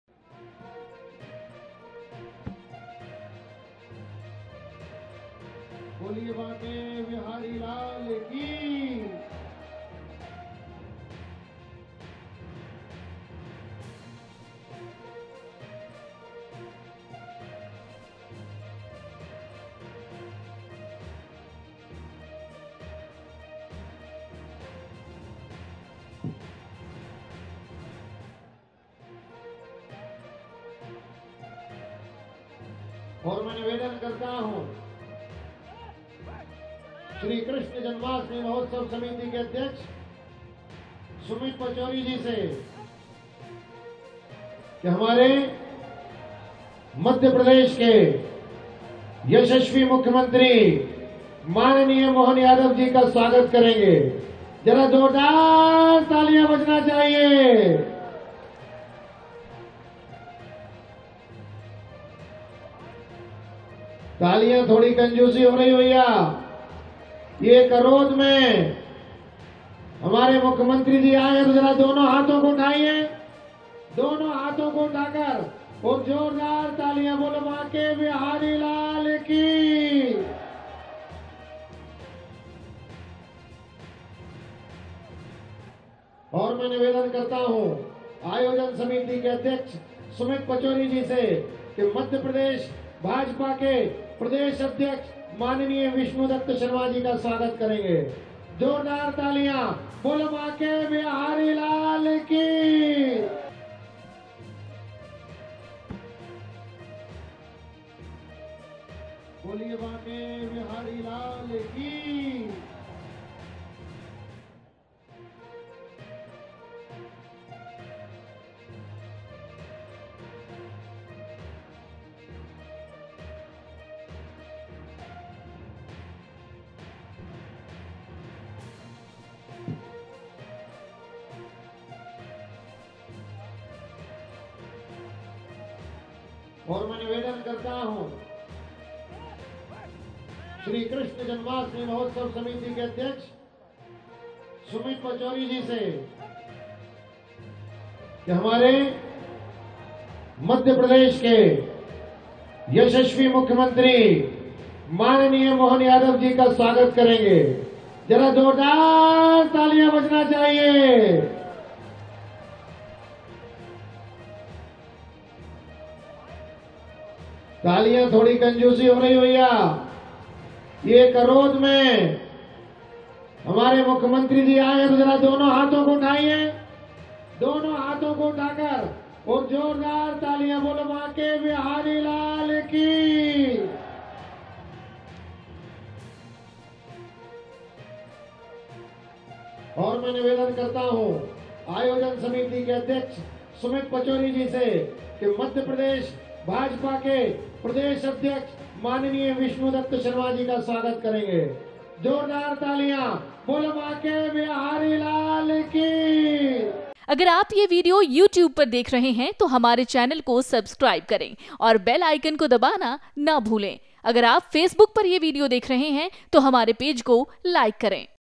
CM Mohan Yadav दही हांड़ी कार्यक्रम में हुए शामिल| Madhya Pradesh | Janmashtami Special |Amar Ujala
Janmashtami Special :CM Mohan Yadav दही हांड़ी कार्यक्रम में हुए शामिल| Madhya Pradesh | Amar Ujala